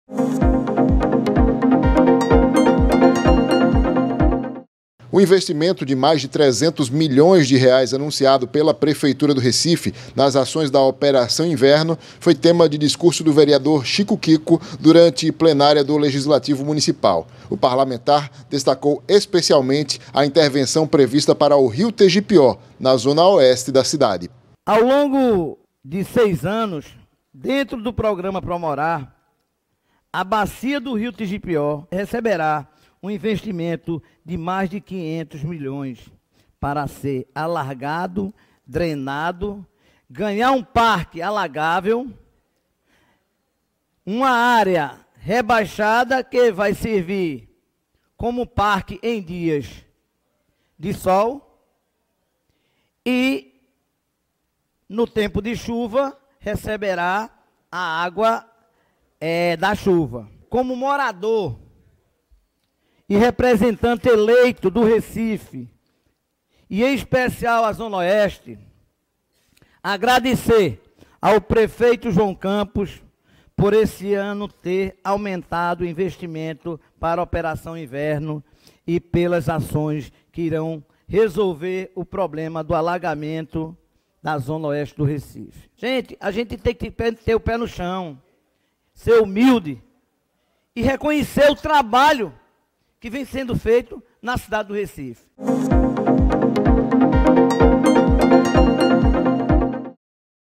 Chico Kiko destaca investimento da Prefeitura na Operação Inverno
O investimento de mais de 300 milhões de reais anunciado pela Prefeitura nas ações da Operação Inverno foi tema do discurso do vereador Chico Kiko, durante plenária do legislativo municipal. O parlamentar destacou especialmente a intervenção prevista para o Rio Tejipió, na Zona Oeste da cidade.